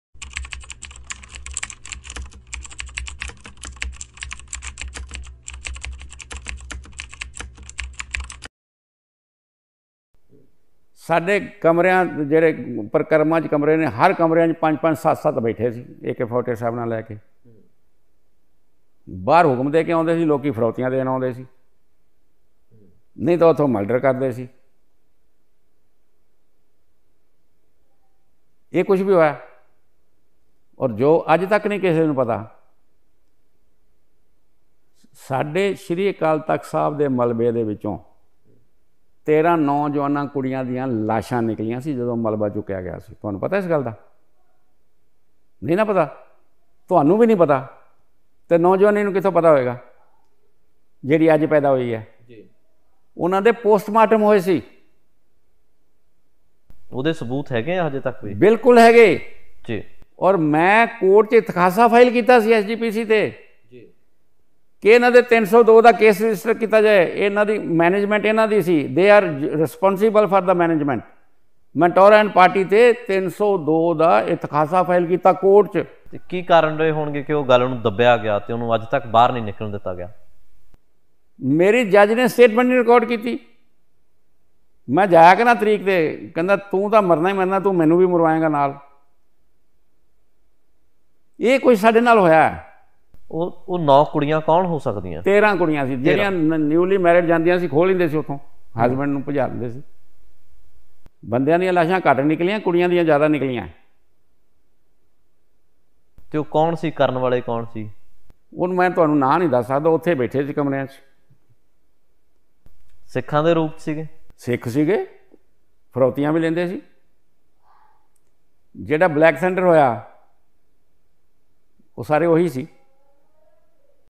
Khalistanis used Golden Temple to abduct women, torture suspects, plan Terrorist acts
Was open secret Khalistanis used GoldenTemple to abduct women, torture suspects, plan tεrrorist acts. But memories so traumatic shameful that even after 3 decades Punjabis are reticent to go there.

Also read @ShekharGupta reportage of BlackThunder.

Important books to read

Harjot Oberoi
W. H. McLeod

Watch my talks & wait for my book.